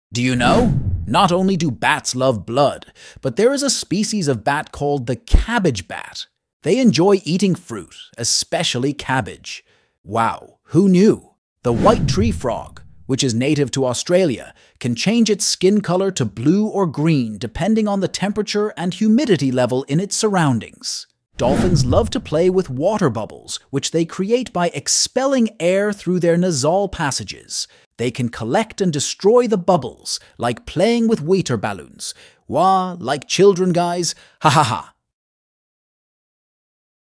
other facts about bats, frogs and dolphins
Not only do bats love blood, but there is a species of bat called the "cabbage bat". They enjoy eating fruit, especially cabbage. The White Tree Frog, which is native to Australia, can change its skin color to blue or green depending on the temperature and humidity level in its surroundings. Dolphins love to play with water bubbles which they create by expelling air through their nasal passages. They can collect and destroy the bubbles like playing with water balloons.